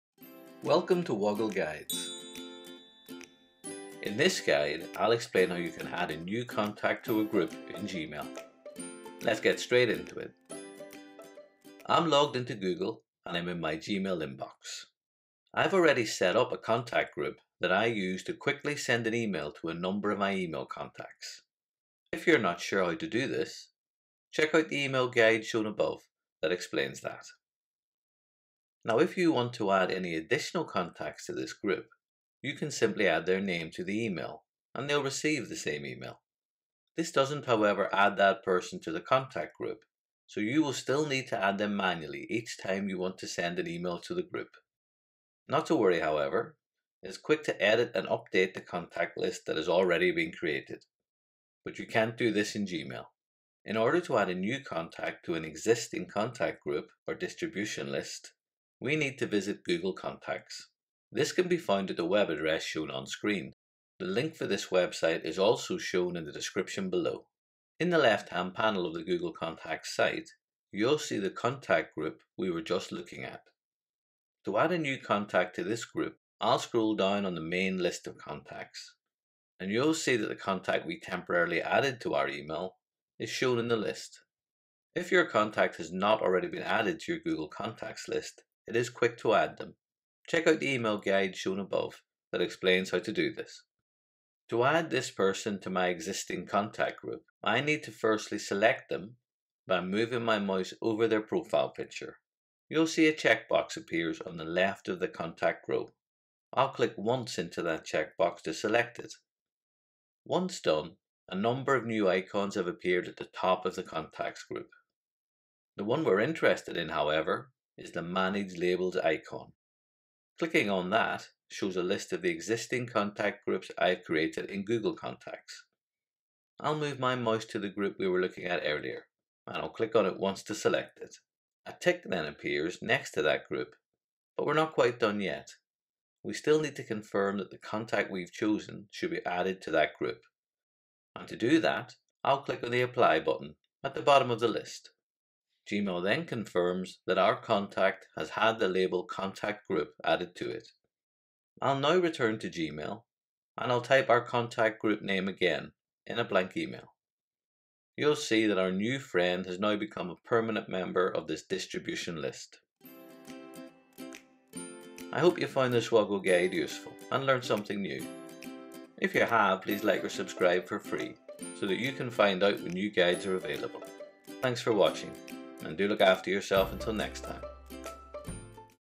How to add contacts to a group in Gmail
In this guide, I’ll explain how you can quickly add new contacts to  a group in Gmail

🕔  Key Moments
00:00 | Introduction
00:12 | How to add contacts to a group in Gmail
02:44 | Wrap up

🔎  Useful links
Find out more about how to add contacts to a group in Gmail in this article from the Google Support Center - ‘’View, group and share contacts’ -